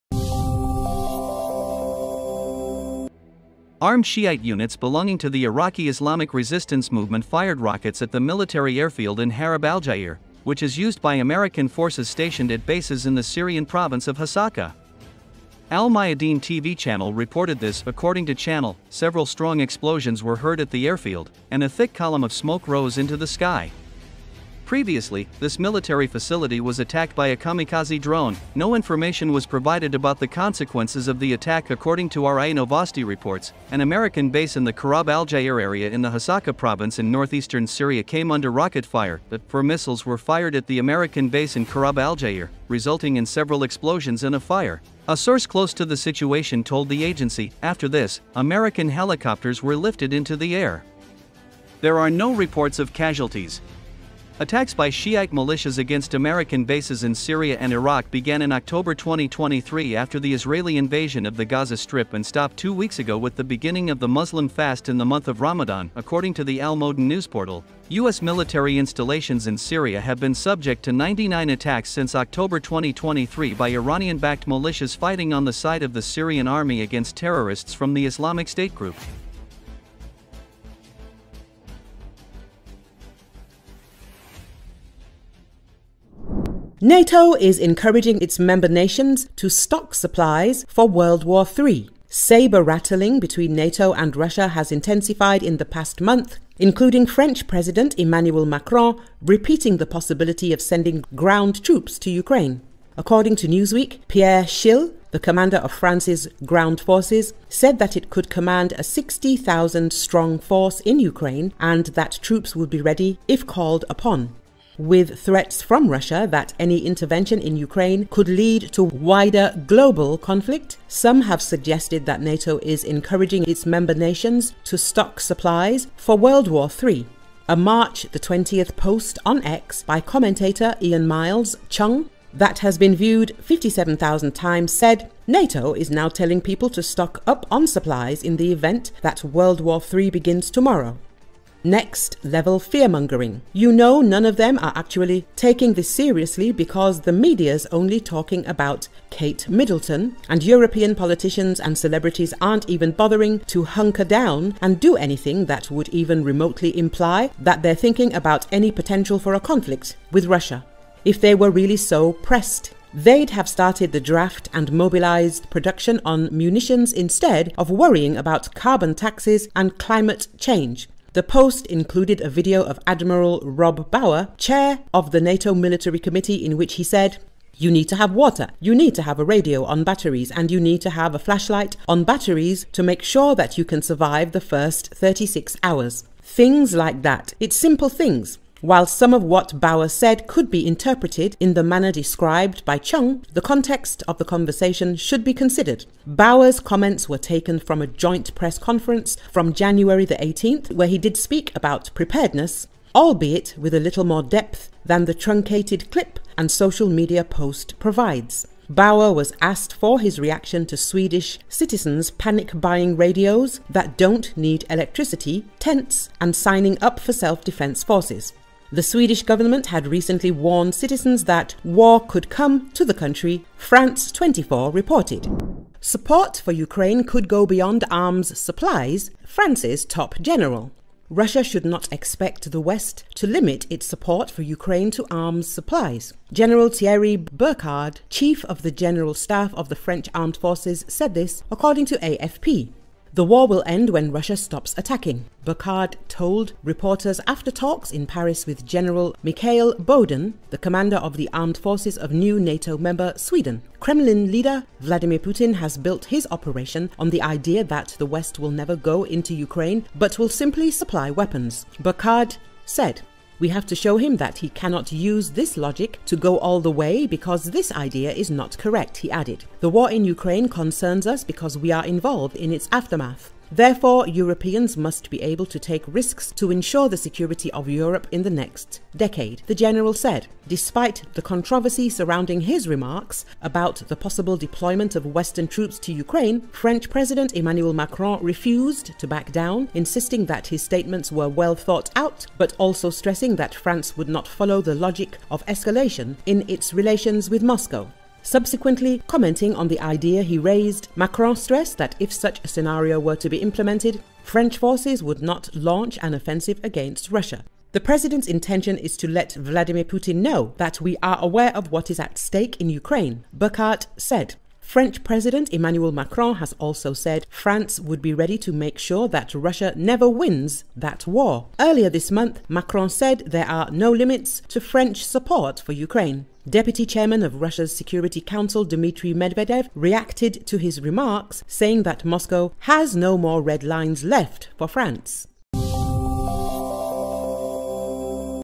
A US military airfield was hit by a missile strike in northeastern Syria
Armed Shiite units belonging to the Iraqi Islamic Resistance movement fired rockets at the military airfield in Harrab al-Jair, which is used by American forces stationed at bases in the Syrian province of Hasakah. Al Mayadeen TV channel reported this.
According to channel, several strong explosions were heard at the airfield, and a thick column of smoke rose into the sky. Previously, this military facility was attacked by a kamikaze drone; no information was provided about the consequences of the attack.
According to RIA Novosti reports, an American base in the Kharab Al-Jair area in the Hasakah province in northeastern Syria came under rocket fire,.
“Four missiles were fired at the American base in Kharab Al-Jair, resulting in several explosions and a fire,” a source close to the situation told the agency.
After this, American helicopters were lifted into the air. There are no reports of casualties.
Attacks by Shiite militias against American bases in Syria and Iraq began in October 2023 after the Israeli invasion of the Gaza Strip and stopped two weeks ago with the beginning of the Muslim fast in the month of Ramadan.
According to the Al Modon news portal, US military installations in Syria have been subject to 99 attacks since October 2023 by Iranian-backed militias fighting on the side of the Syrian army against terrorists from the Islamic State group.